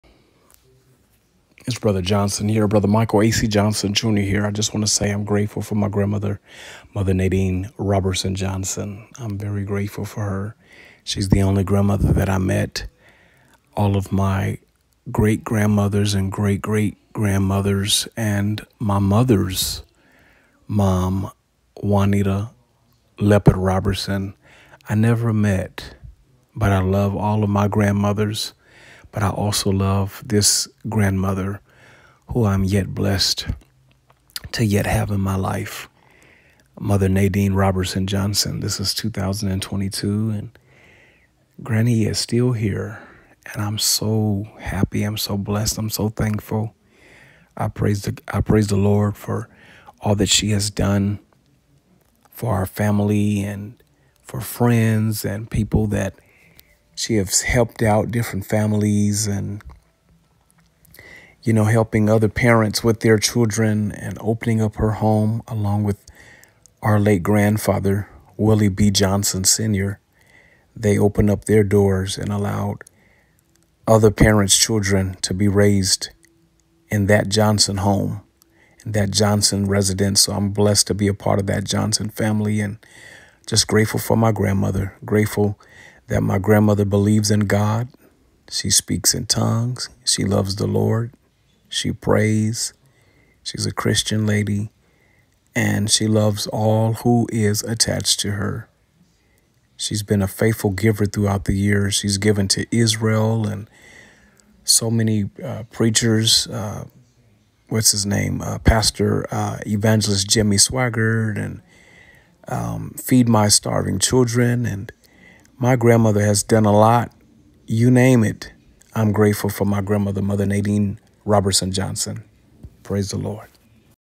Mother Nadine Roberson-Johnson
Evangelist (Brother) Michael A.C. Johnson, Jr. Thanking God for His Grandmother, Mother Nadine Roberson-Johnson..